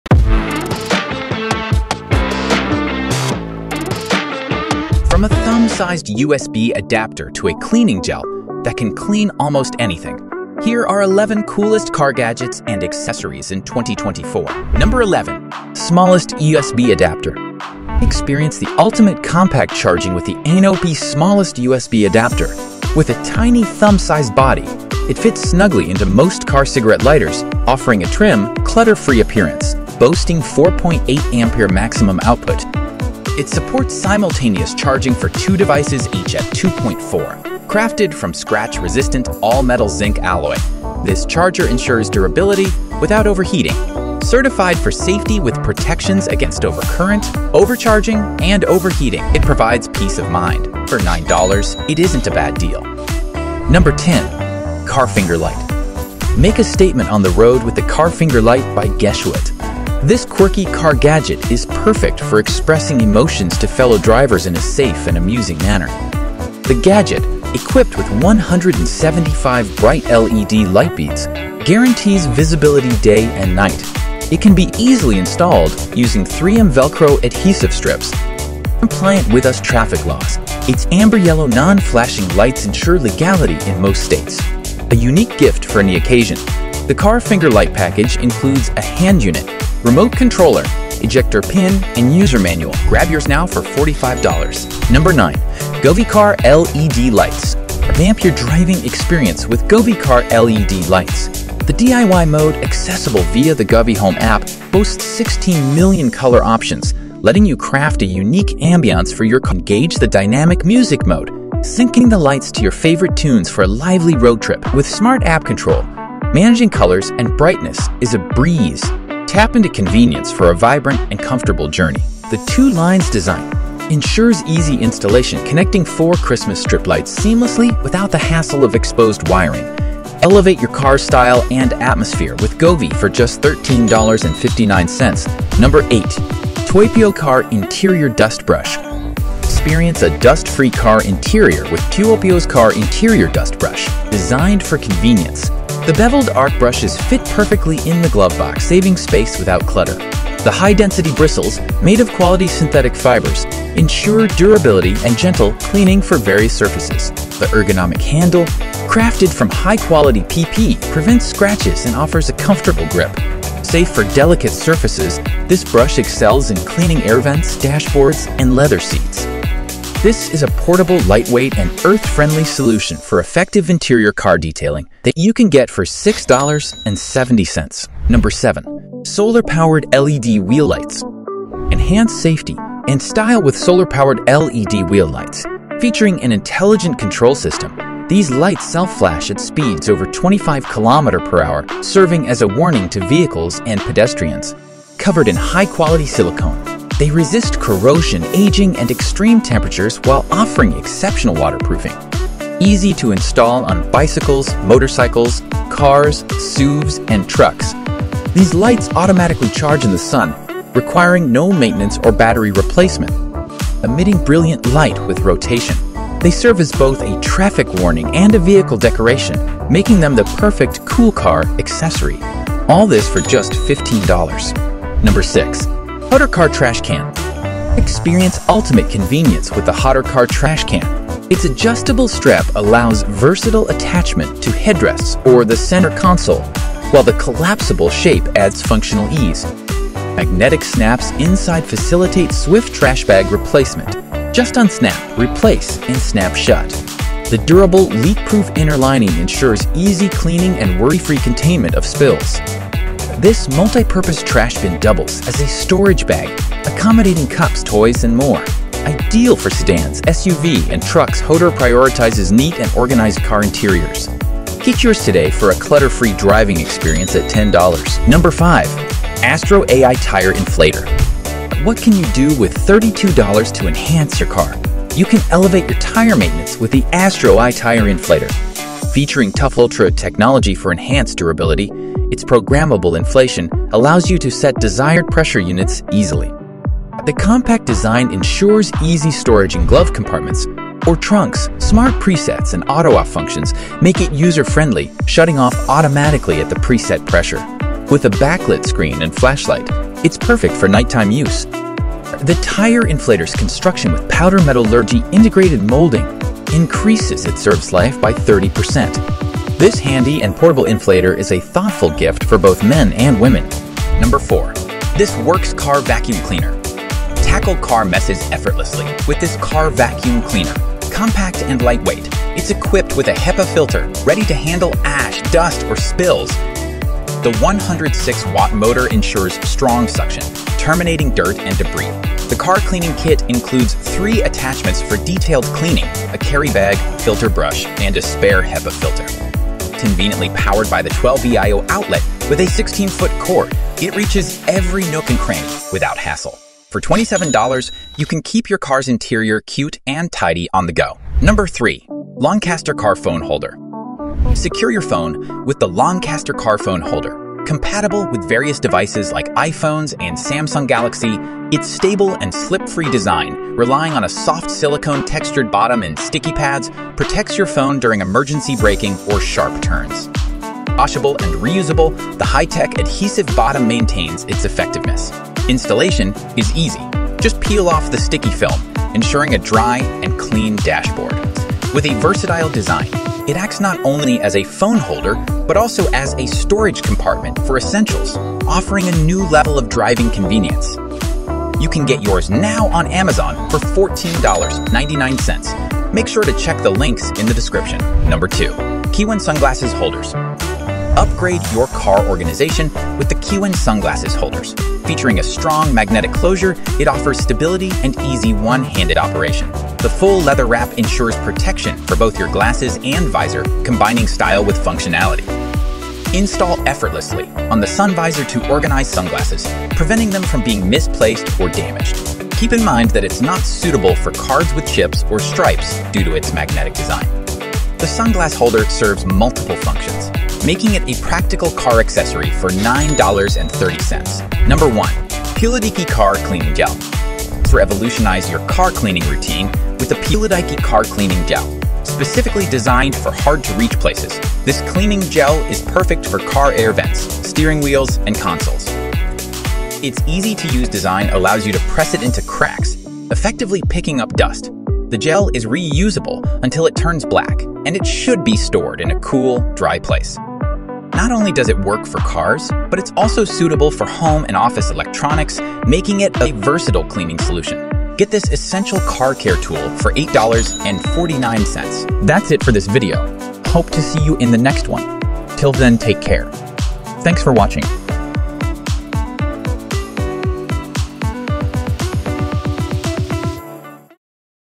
11 COOLEST CAR GADGETS AND ACCESSORIES IN 2024 ▶▶
11 COOLEST CAR GADGETS AND ACCESSORIES IN 2024 ▶▶

From the tiniest USB adapter to solar-powered LED wheel lights, elevate your driving experience with cutting-edge technology. Explore unique finds like the Car Finger Light, Govee Car LED Lights, and Hotor Car Trash Can. Enhance safety with Solar-Powered LED Wheel Lights and stay organized with the Loncaster Car Phone Holder. Upgrade your car organization with Kiwen Sunglasses Holders and revolutionize your cleaning routine with PULIDIKI Car Cleaning Gel. Don't miss out on these must-have accessories! 🚗💡

11. Smallest USB Adapter
10. Car Finger Light
9. Govee Car LED Lights
8. TEUOPIOE Car Interior Dust Brush
7. Solar-Powered LED Wheel Lights
6. Hotor Car Trash Can
5. AstroAI Tire Inflator
4. ThisWorx Car Vacuum Cleaner
3. Loncaster Car Phone Holder
2. Kiwen Sunglasses Holders
1. PULIDIKI Car Cleaning Gel

📢 Don't Forget!